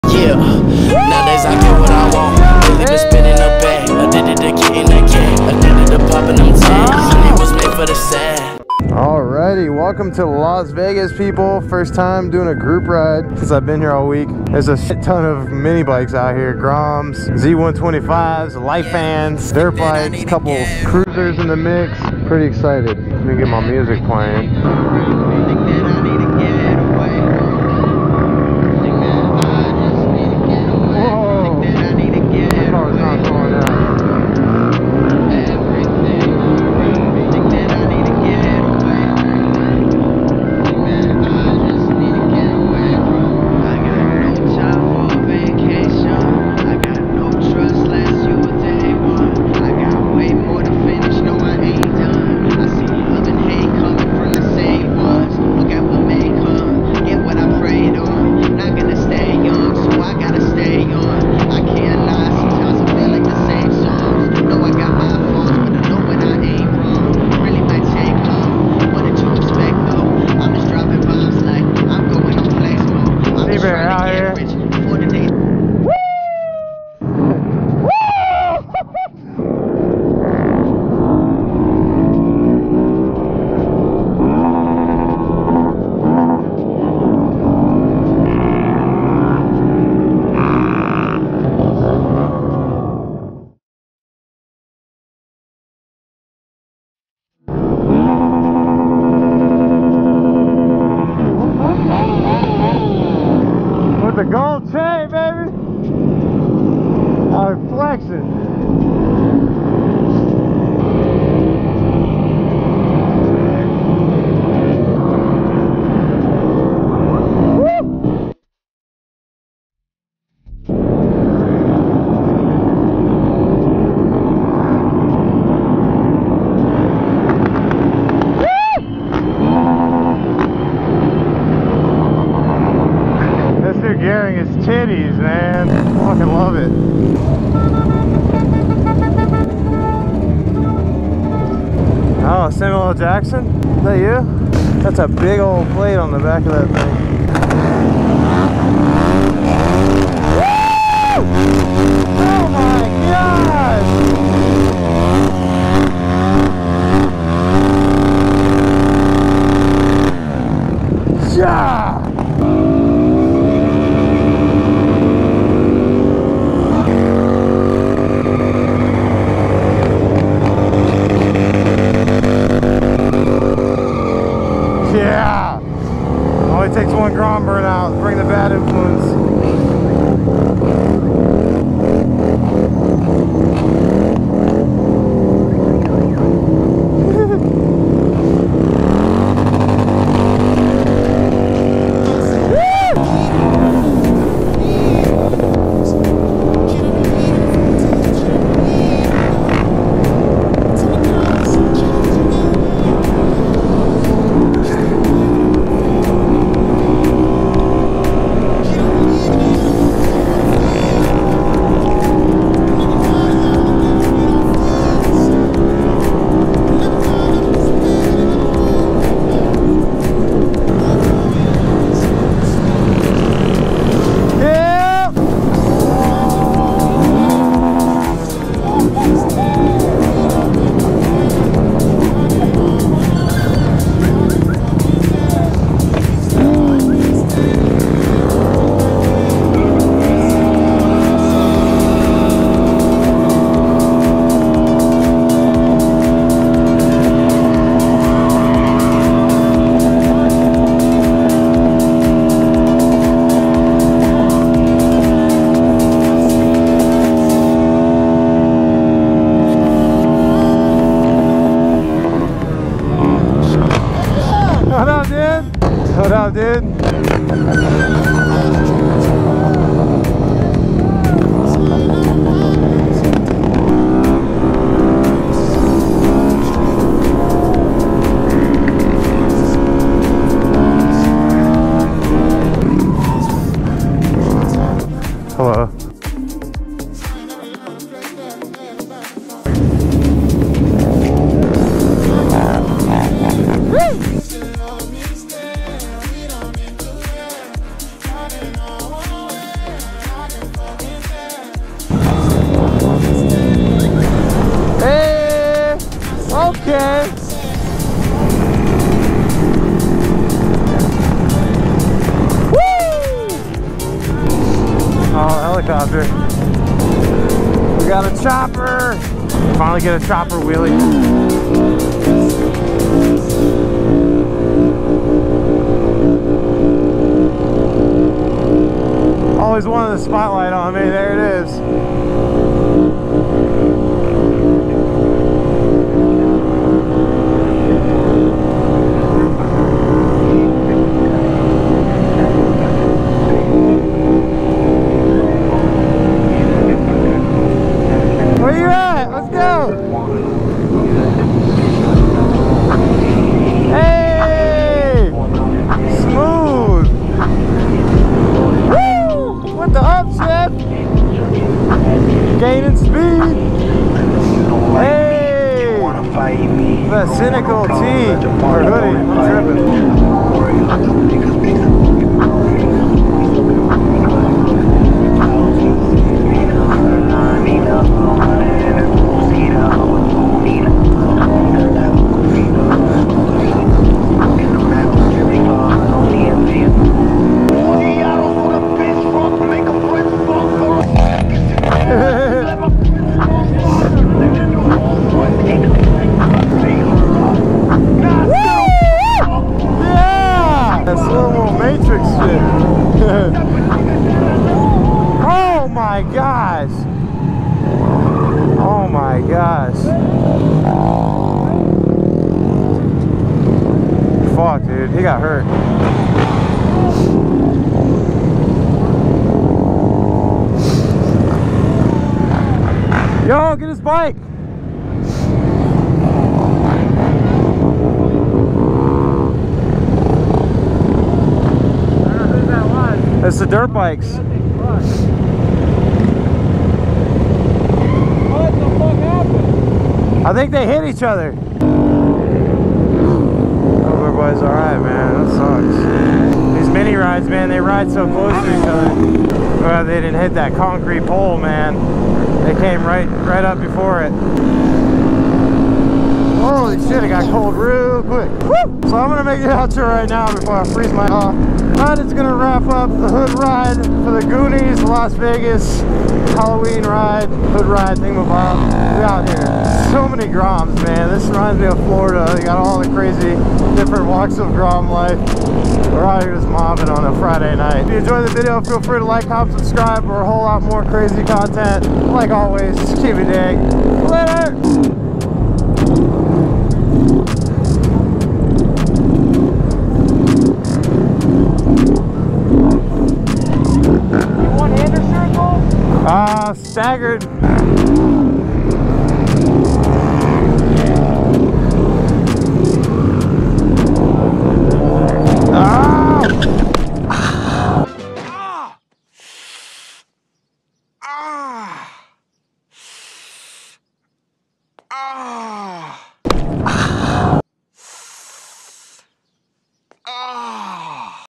Vegas Bikes Go Hard! - Little Bike Pile Up
I took my 2020 Honda Grom to Las Vegas for the annual Gooniez Grom Squad Halloween ride, there were a ton of different mini bikes out there as well as a police helicopter giving us coverage from above.